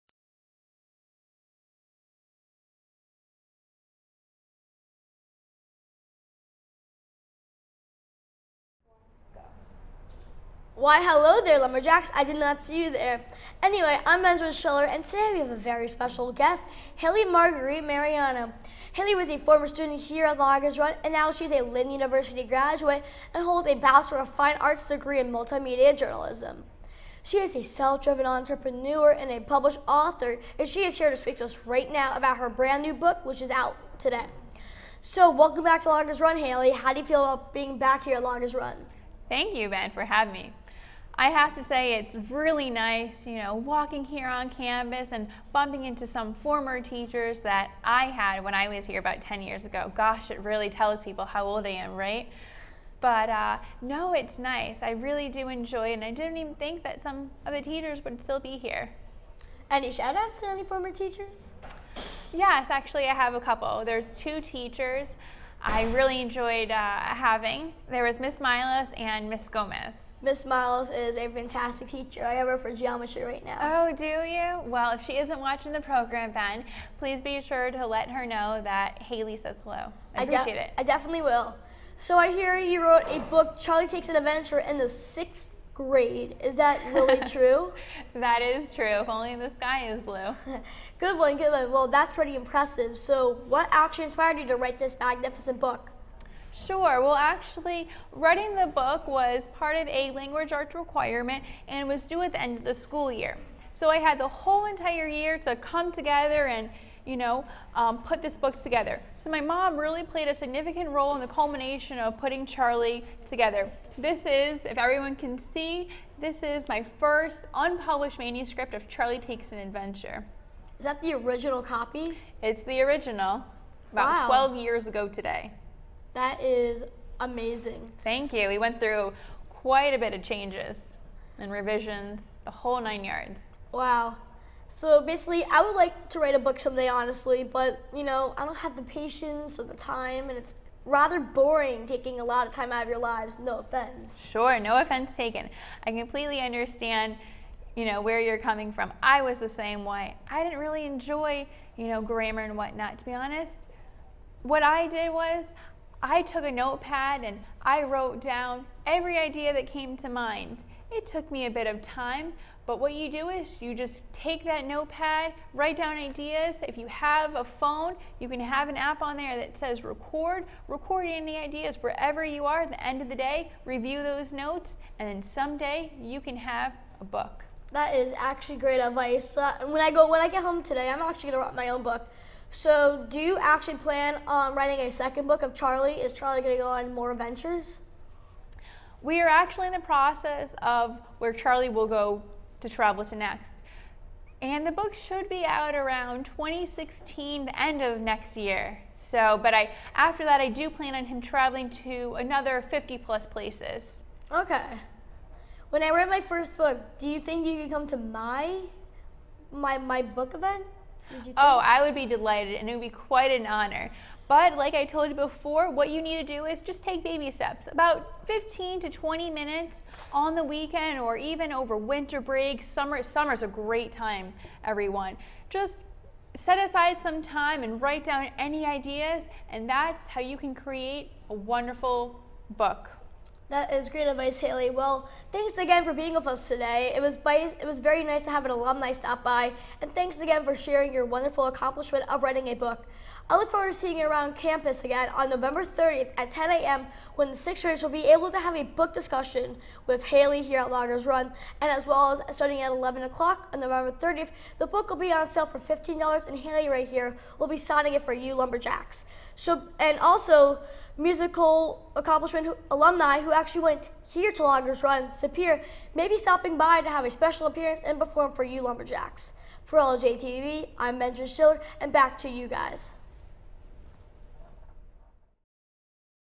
Interview at Loggers' Run Middle School with 8th grade student, Benjamin Schiller
Charlie Takes an Adventure book interview with anchor Benjamin Schiller